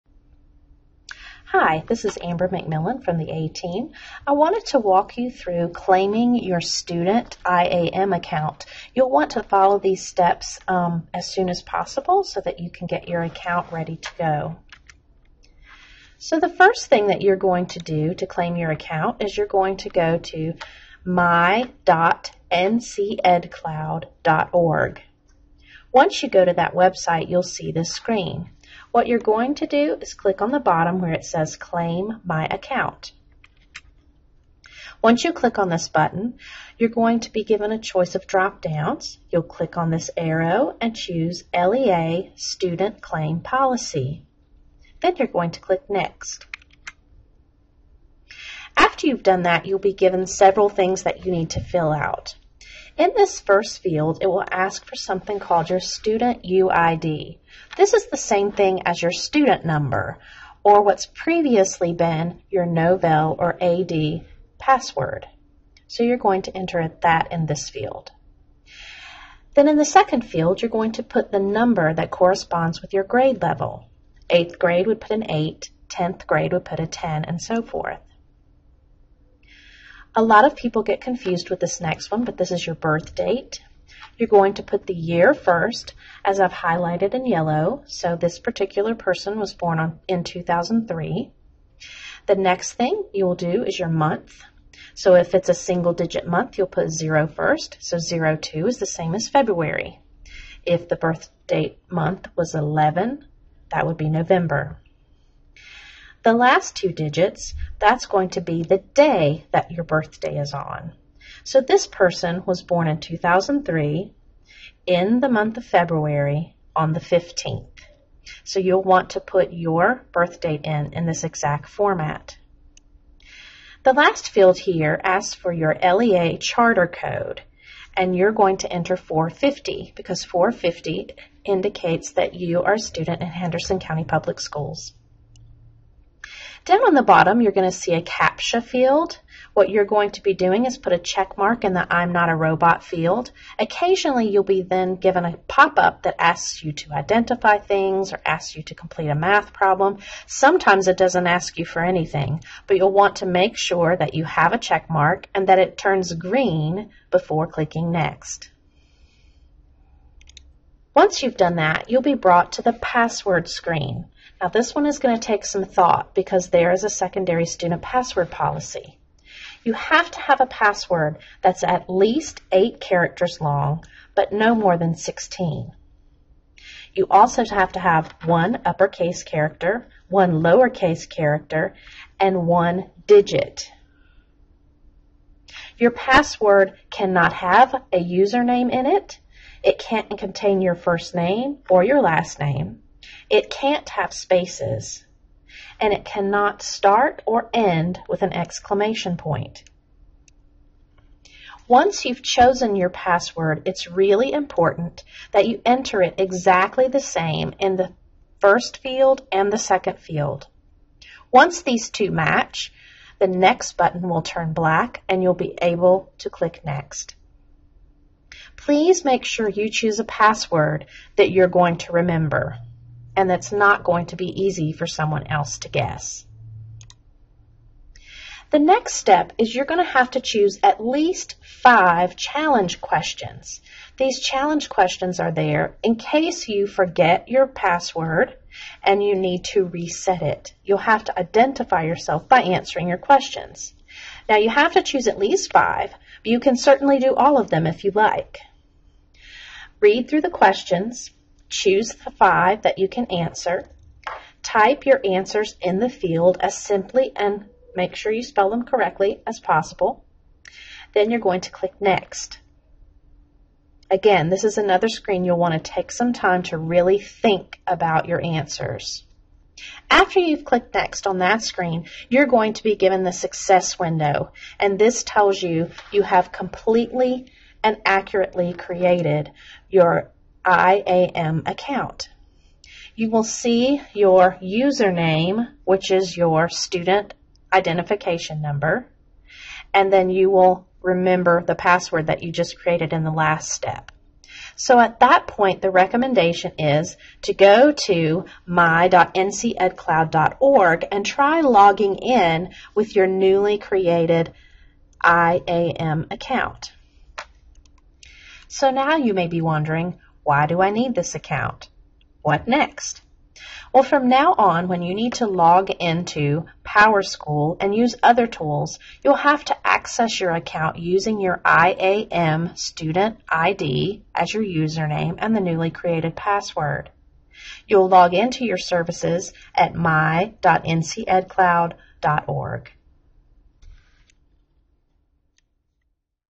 Claiming Your Student IAM Account
This video is for students who are claiming their IAM accounts.